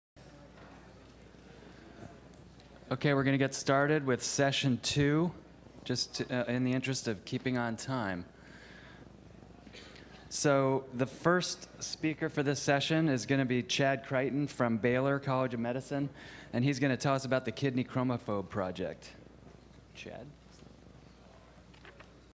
Session 2 Introduction - Josh Stuart
May 12, 2014 - The Cancer Genome Atlas 3rd Annual Scientific Symposium